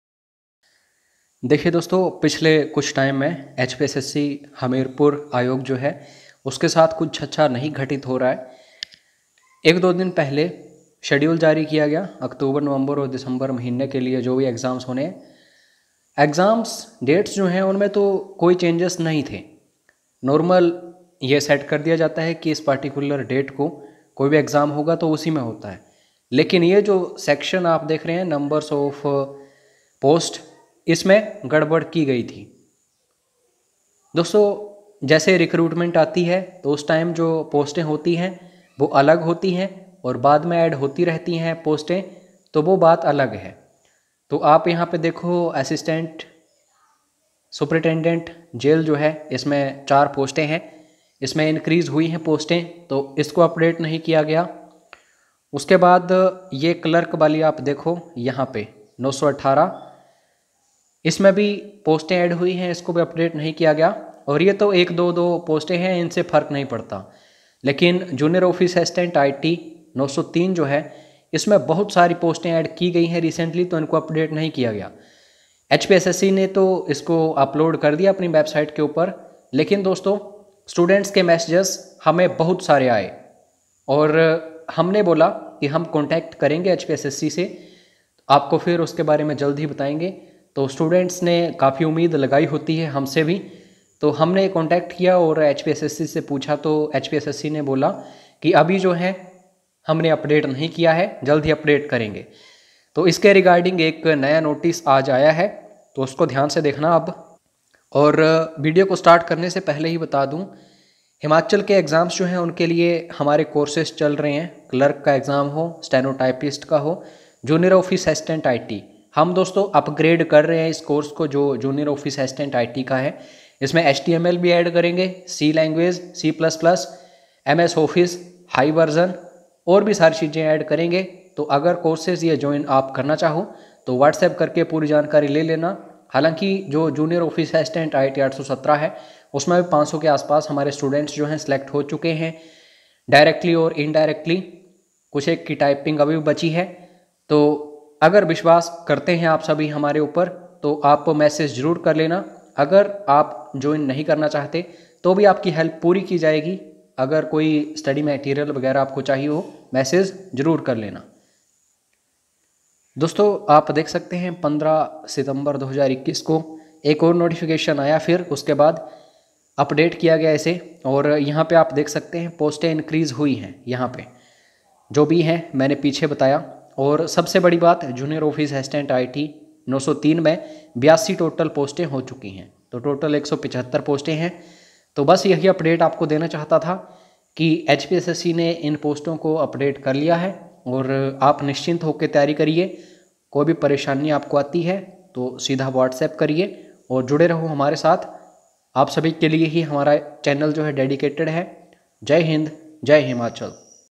HPSSC HPSSSB Revised Schedule for October, November, December Exams (New Exam Date Sheet 2021)
सारे Updates के लिए

और

जरूर JOIN करें।

धन्यवाद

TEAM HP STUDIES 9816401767